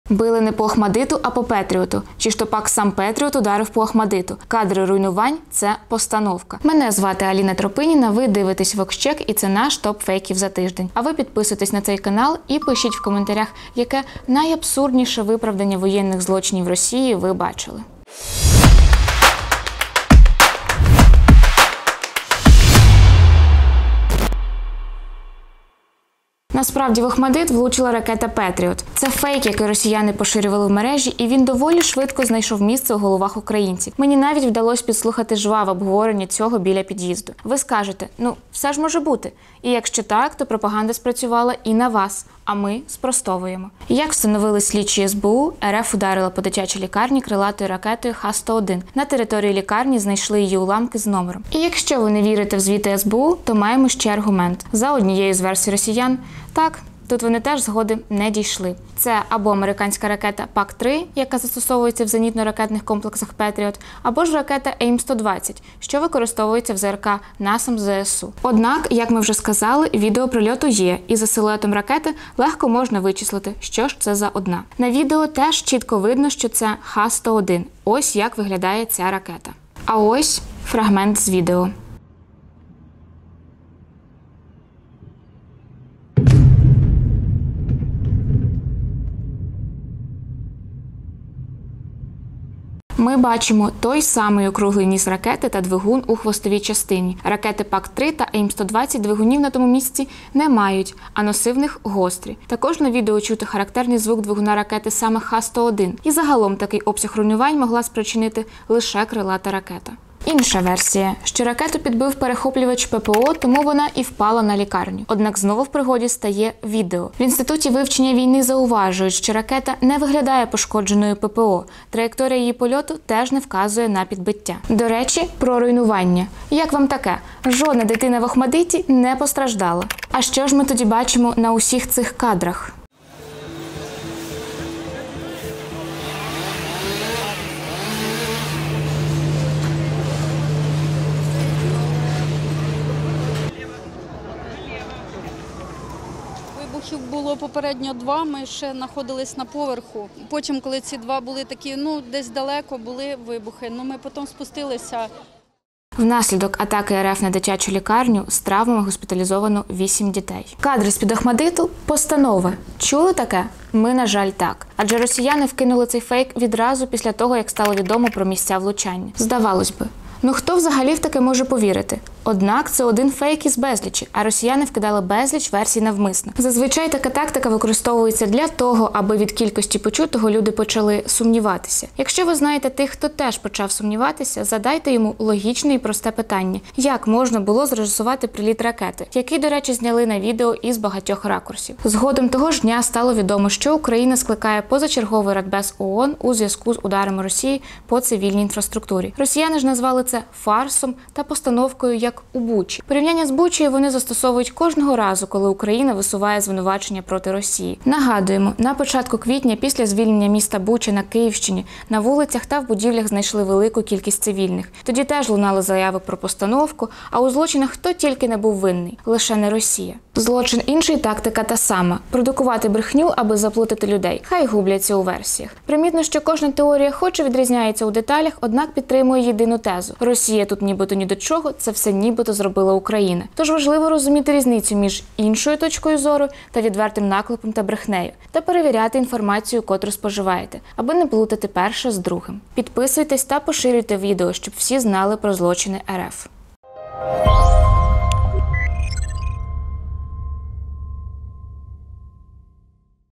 Як росіяни виправдовують обстріл Охматдиту? | ТОП фейків
Вранці 8 липня російська армія завдала масованого удару по Україні. Під удар потрапили Київ, Кривий Ріг, Дніпро, Слов’янськ, Краматорськ та інші міста. Станом на 11 липня відомо про 44 загиблих, понад 190 людей отримали поранення. Цілились по заводах, житлових будинках та лікарнях. Традиційно, масовані російські атаки супроводжуються хвилею абсурдних теорій та виправдань, аби приховати власні злочини, замулити очі аудиторії, створити «альтернативну версію», яку подаватимуть в іноземних новинах як «позицію російської сторони».

У цьому випуск про:
00:00 - вступ
00:27 - в Охматдит влучила ракета Patriot
01:51 - ракету підбив перехоплювач ППО
02:07 - жодна дитина в Охматдиті не постраждала
02:41 - кадри з Охматдиту — постановка